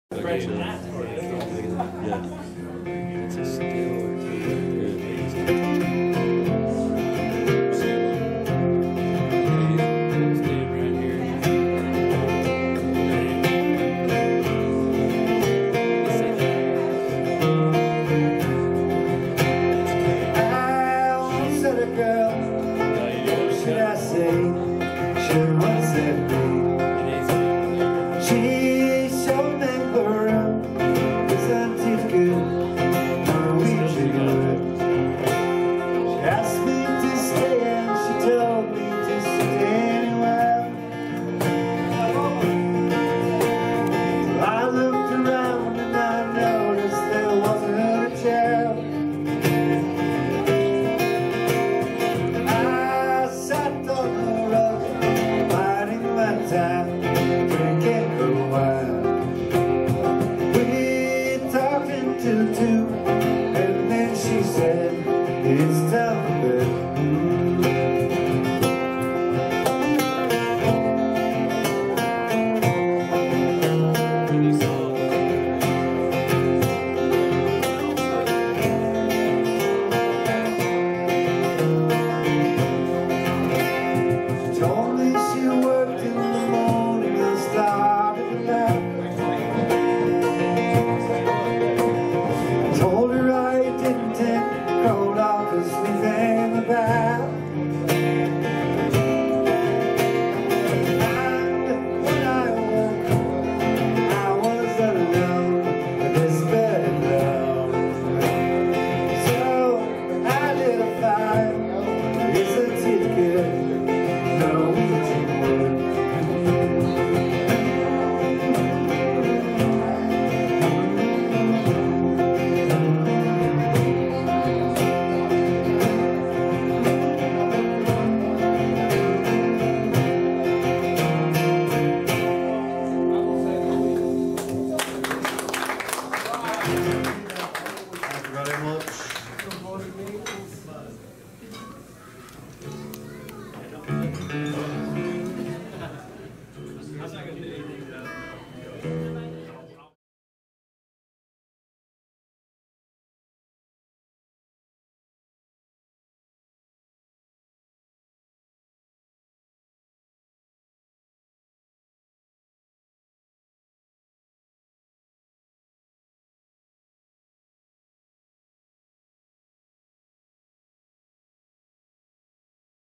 Isayah Warford and Jeff Stephenson
CaptureCrew llc gettin' some video of friends jammin' live at Mojos Pints and Pies